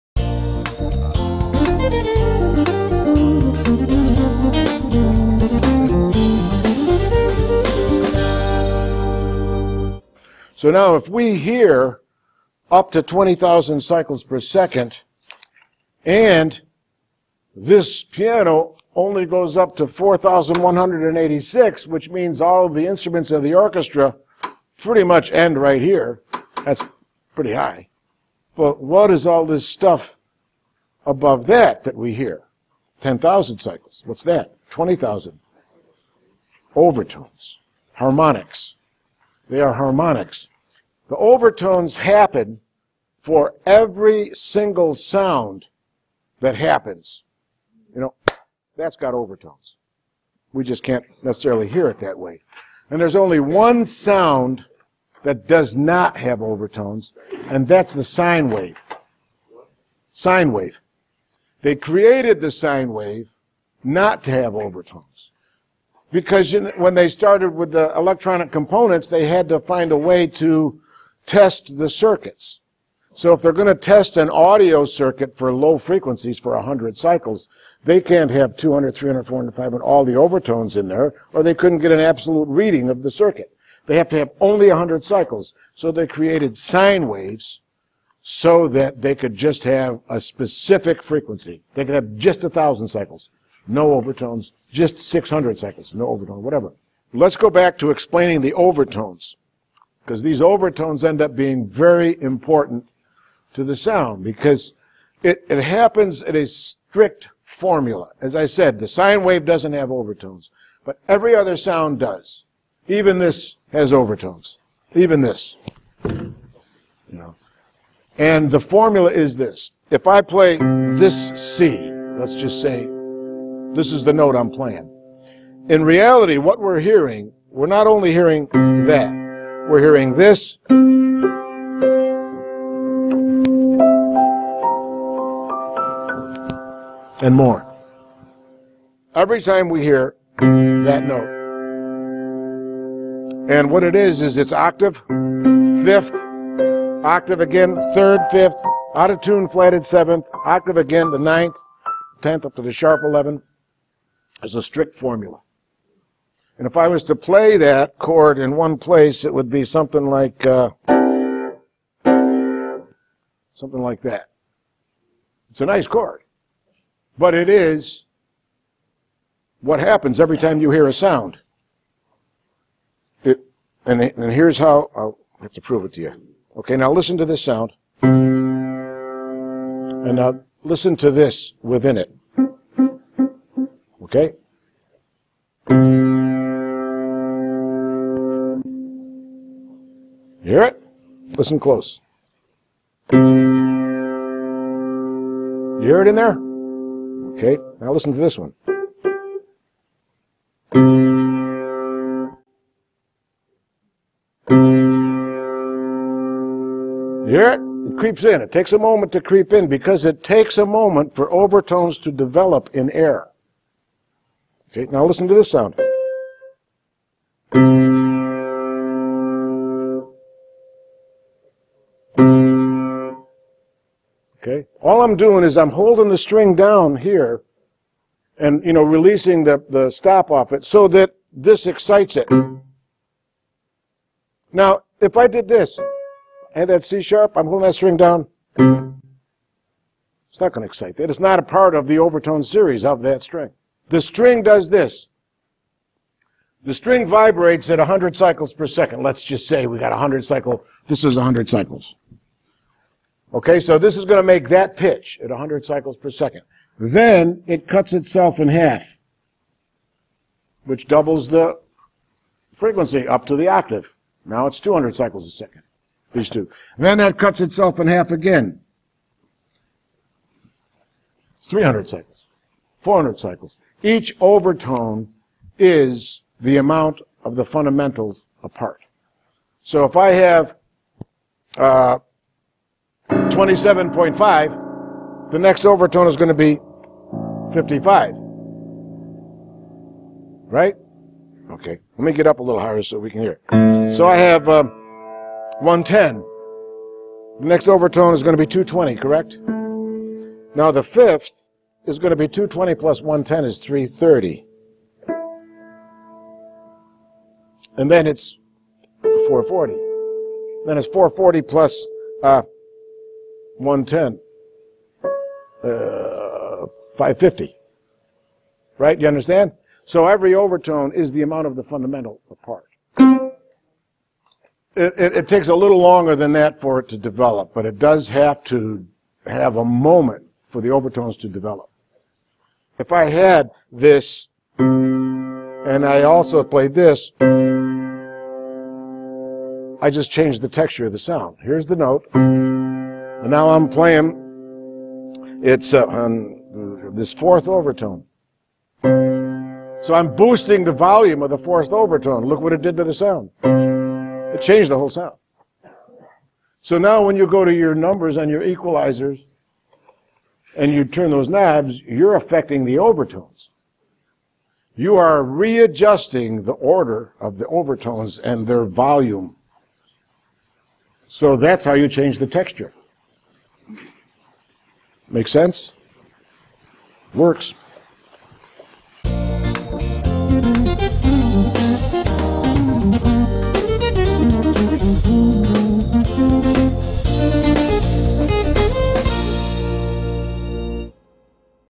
What are Overtones?
"What are Overtones?" is a lecture video from the textbook Basic Audio Recording Techniques, taught by Mike Julian at Los Angeles Valley College.